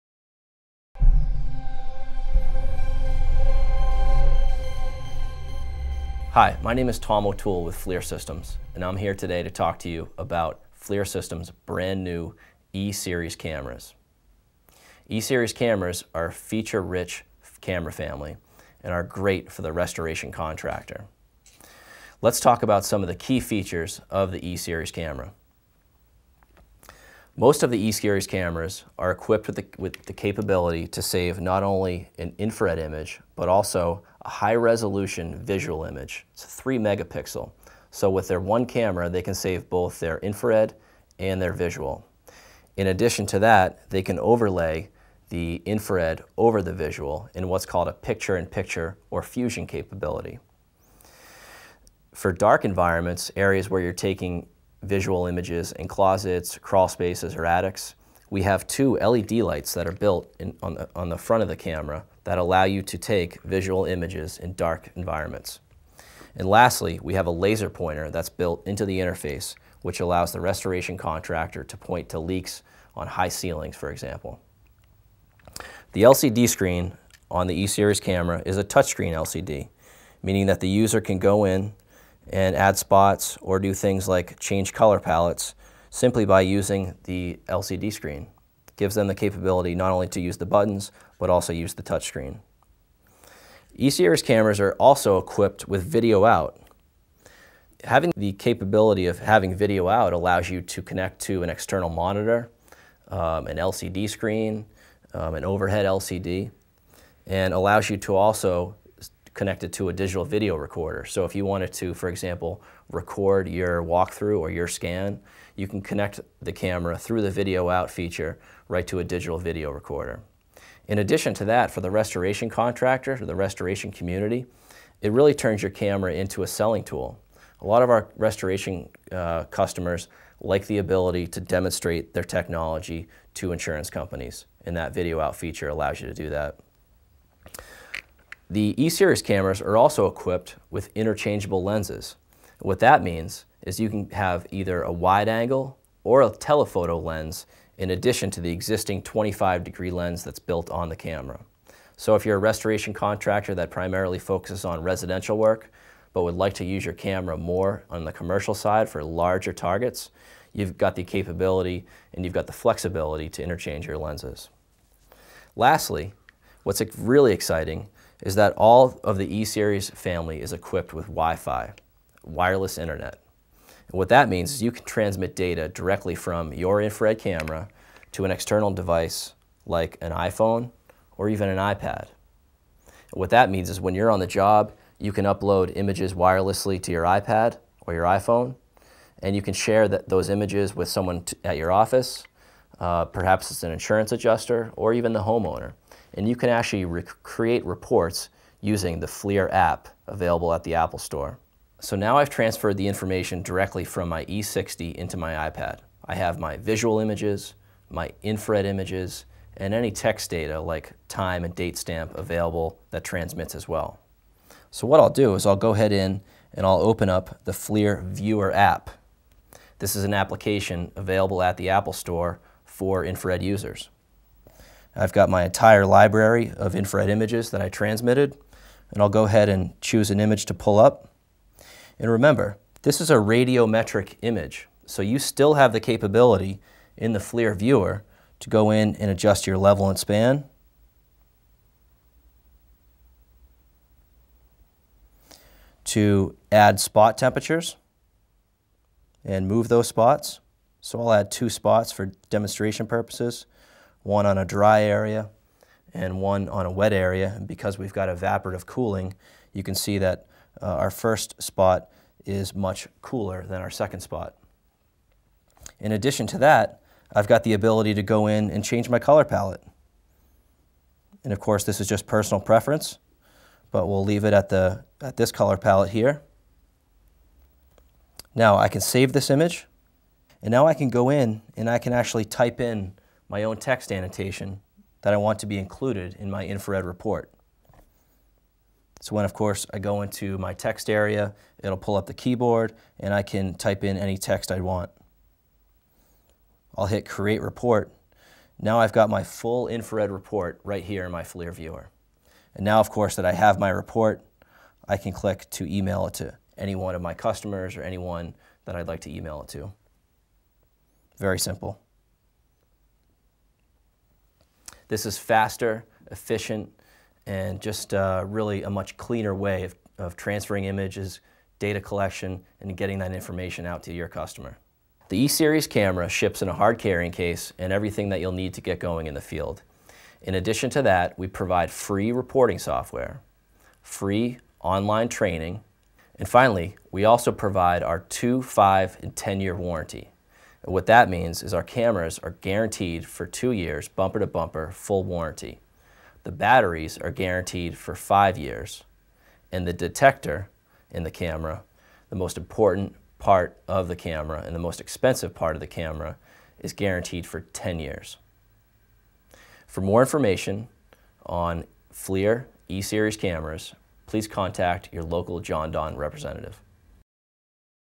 Flir E Series Cameras - Jon-Don Video
The E Series Infrared cameras from Flir offer high resolution IR and standard imaging. These cameras feature digital video out as well as the ability to store regular images, IR images, and meta data including temperature, time, and other important details on an SD card. Data can also be transmitted to an Iphone, Ipad, or computer. This is an introductory video reviewing the features and benefits of these cameras.